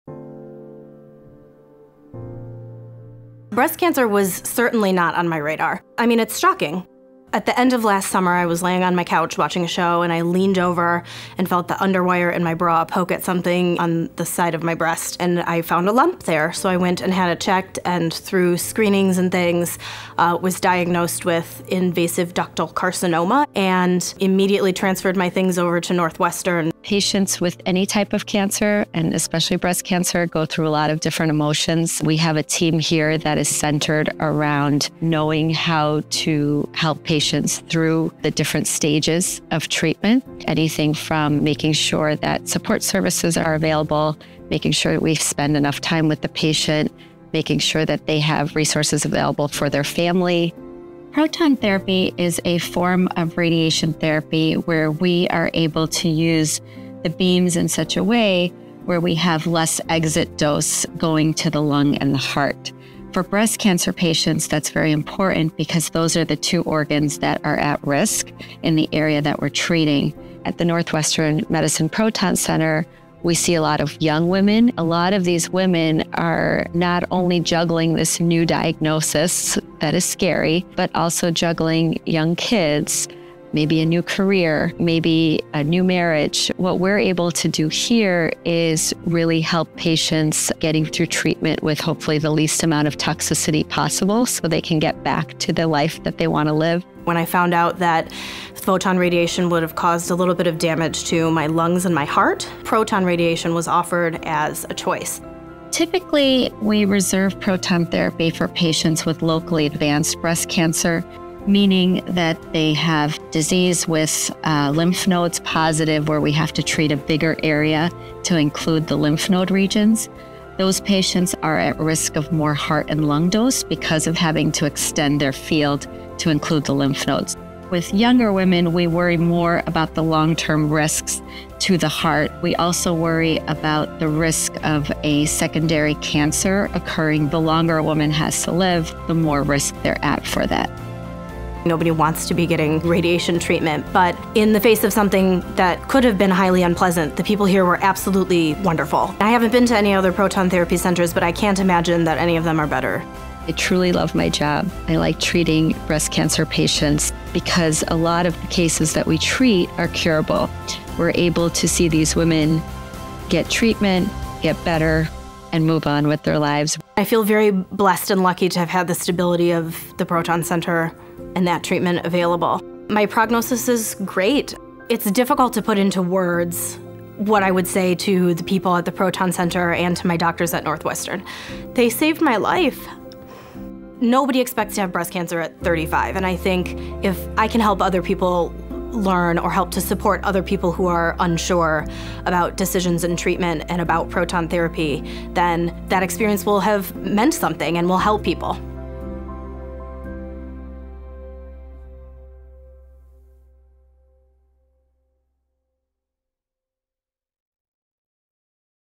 Proton Therapy for Breast Cancer
As a patient with breast cancer, Katie was looking for a treatment that would limit the amount of radiation to her lungs and heart. The Northwestern Medicine Proton Center team treated her cancer with state-of-the-art proton therapy with a successful outcome. Katie says, “I feel very blessed and lucky to have had the stability of the Proton Center and this treatment available.” Learn more about how proton therapy can help patients with breast cancer.